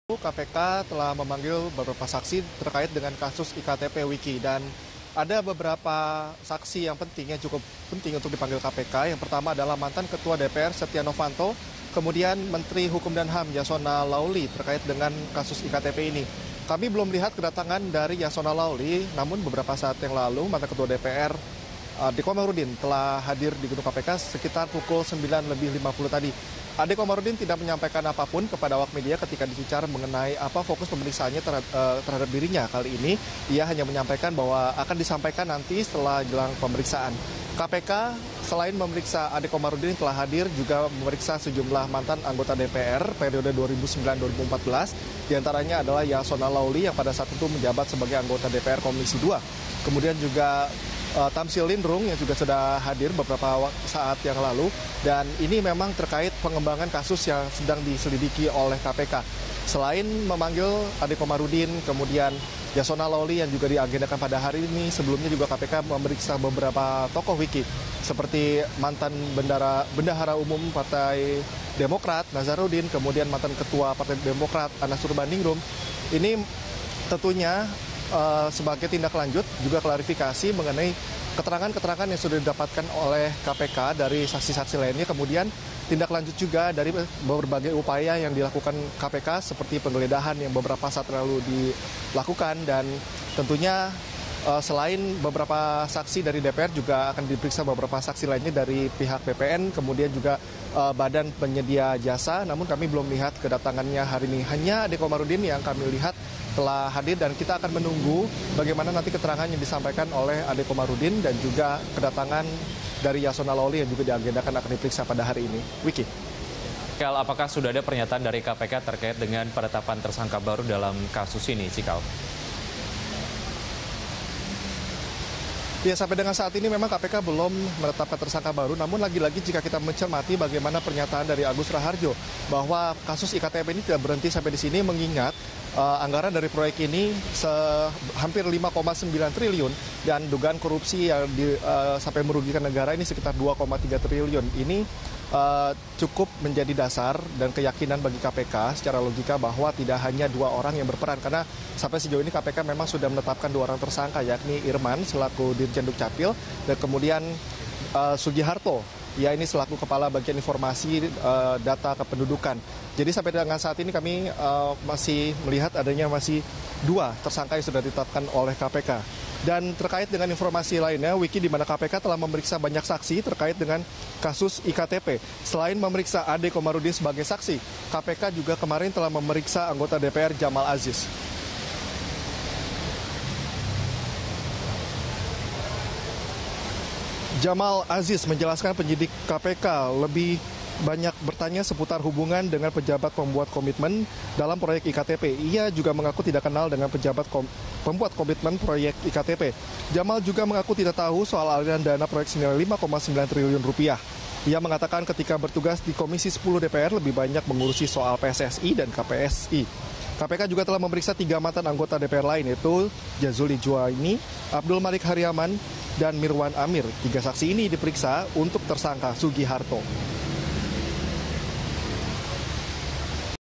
KPK Periksa Mantan Ketua DPR Terkait Kasus Korupsi dana E-KTP
Penyidik Komisi Pemberantasan Korupsi (KPK) memeriksa Menteri Hukum dan HAM (Menkumham), Yasonna H Laoly dan mantan Ketua DPR, Ade Komarudin, Jumat (3/2).